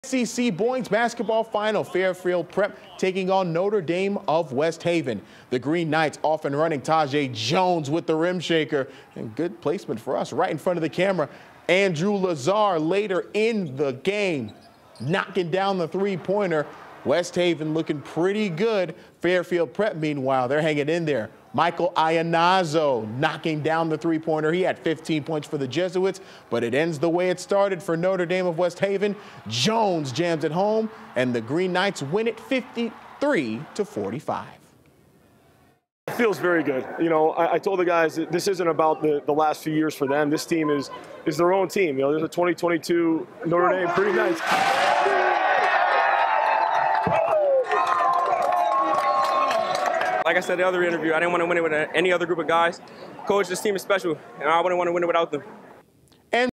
Which Connecticut high school boys basketball team won SCC championships?
Notre Dame High School of West Haven defeated Fairfield Prep, winning the SCC boys final.